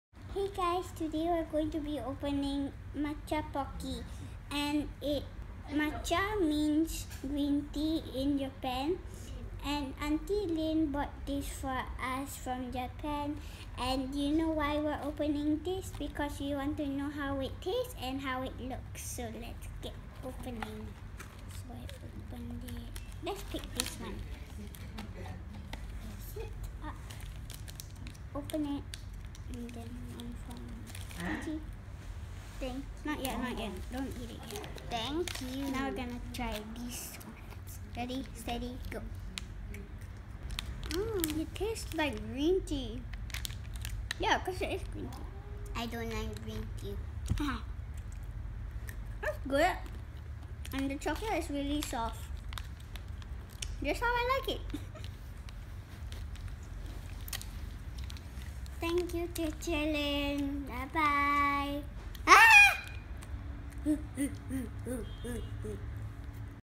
Matcha Pocky Review
Jojo and Gigi just got special treat from aunty Lynn whom just came back fr Japan: Matcha Pocky! Let's see the girls reaction to that😊